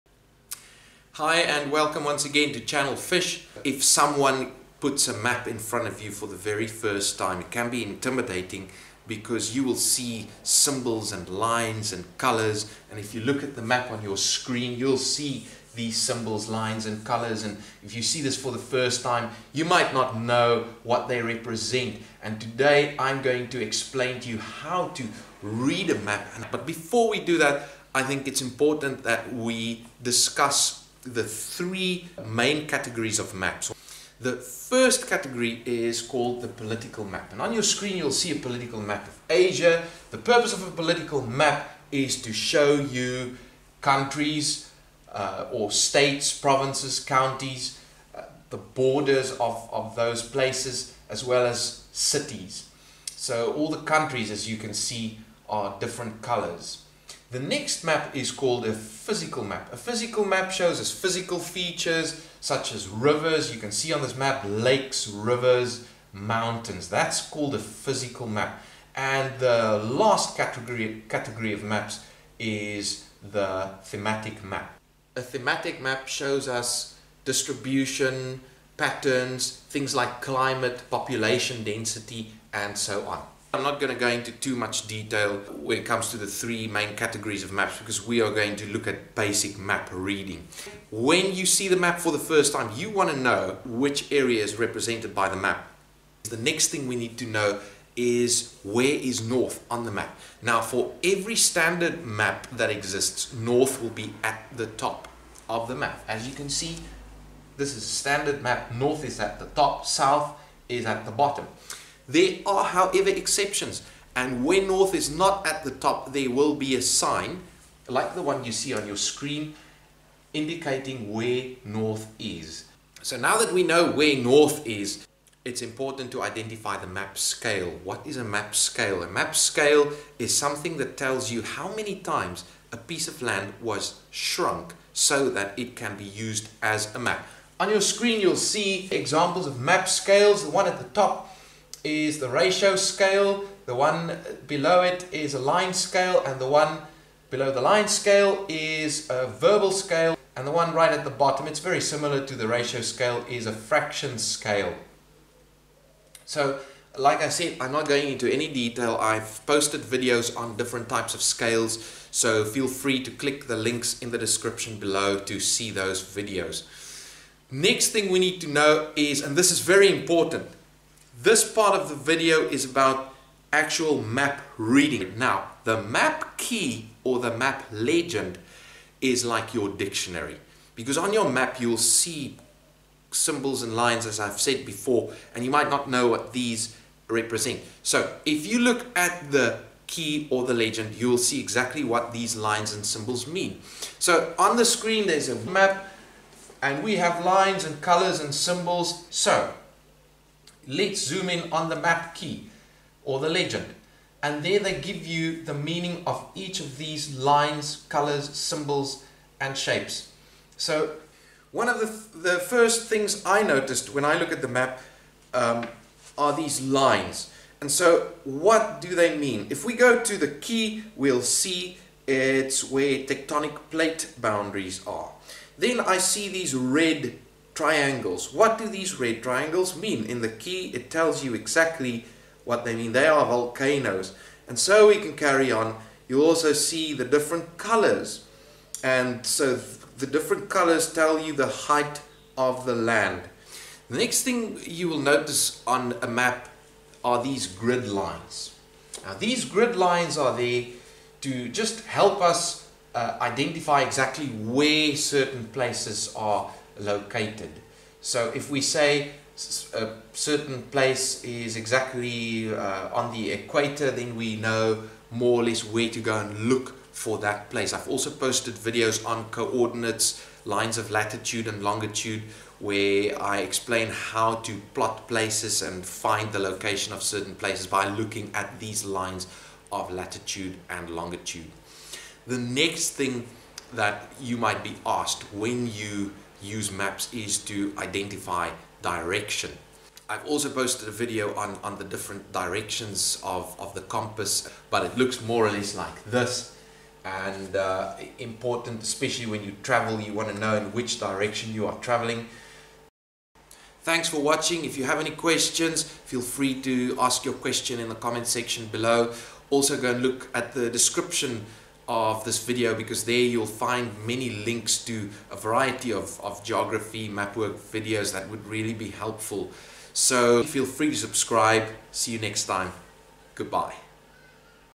How to read a map
Geography mapwork: How to read a map. This video is about basic map reading skills, which include the map key, direction and the compass rose, where to find North, map scale, physical and political maps, map key symbols, etc. If you want more detail, click the links below.